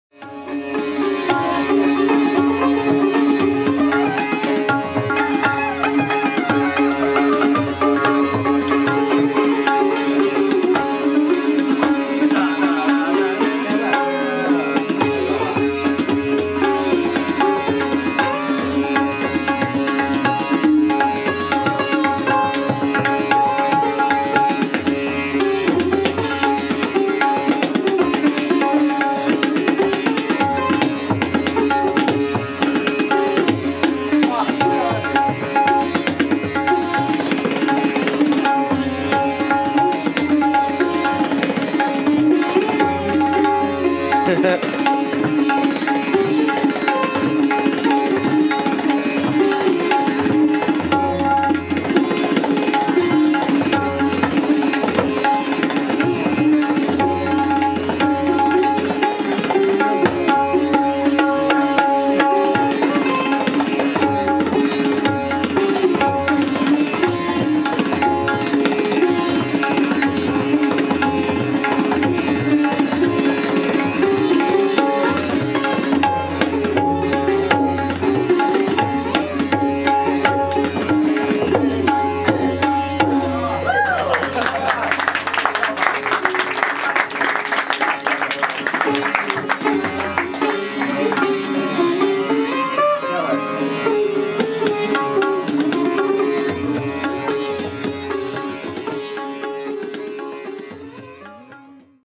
Paul and Pandit Shashanka Bakshi
Raga Puriya Dahnashree

Paul Livingstone - sitar
Pandit Shashanka Bakshi - tabla